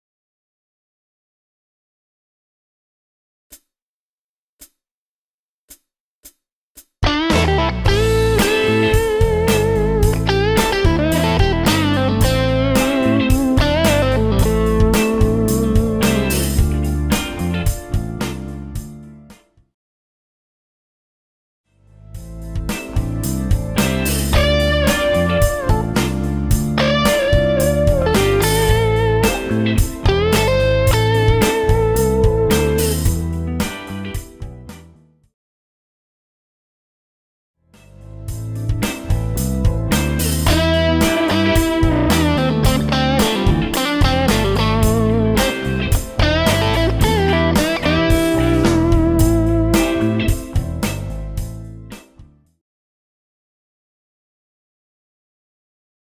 Blues Lesson: Solo
Blues Guitar Lesson:hot guitar solo's in this free video clip on how to play blues guitar.